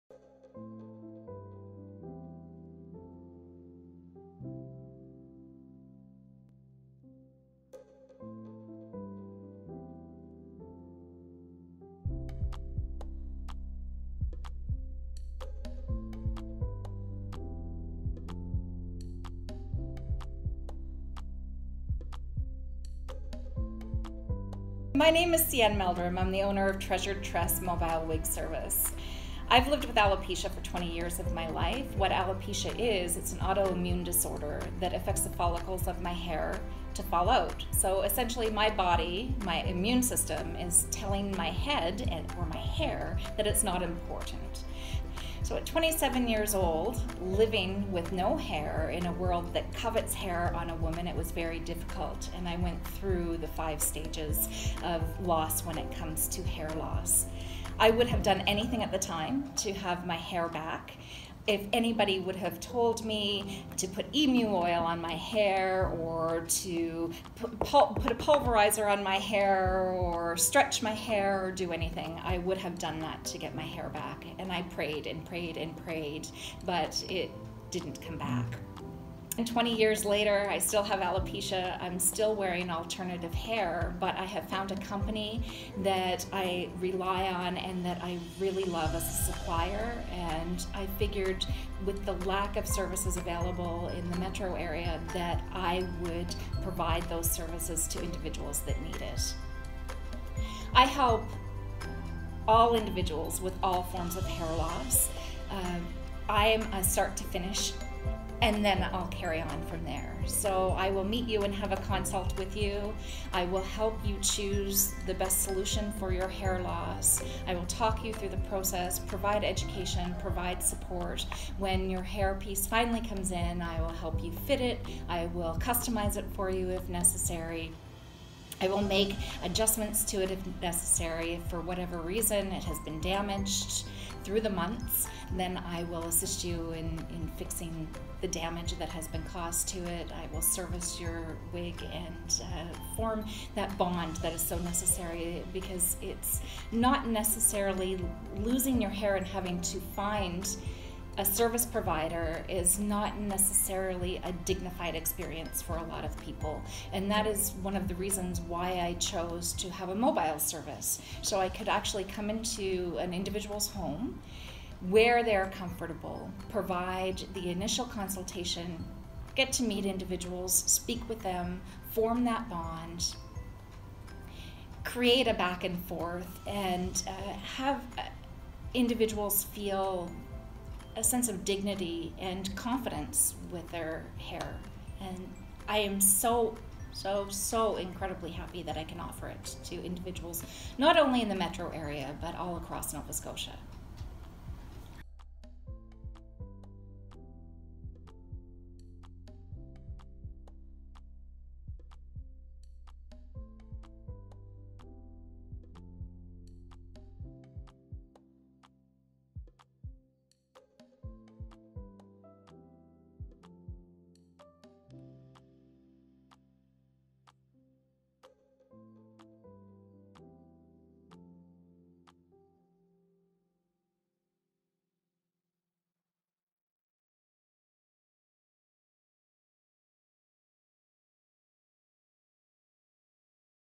CUA Member Treasured Tress Mobile Wig Service
Seanne Meldrum brings a unique element to HairstylistAppreciationDay. Since being diagnosed with Alopecia at age 25, she has experienced first hand the struggles on the journey through hair loss. With her business Treasured Tress Mobile Wig Service, Seanne brings comfort and confidence directly to your front door. Some would even say, she touches more hearts than hair. 👧👩👵👱‍♀️